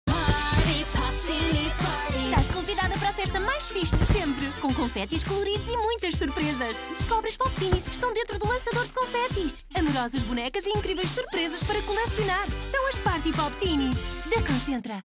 Party Pop Teenies
Bem vindos ao mundo de Party Pop Teenies: onde todos os dias são uma festa e estão todos convidados!! Há tantas surpresas e tanto para festejar! Que surpresa estará dentro deste lançador de confettis? Puxa a patilha, roda o lançador e… Surpresa!!! Descobre qual é a tua Boneca Party Pop Teenies numa explosão de conffetis colecionáveis, e na parte inferior do lançador, tens acessórios para juntares à tua festa!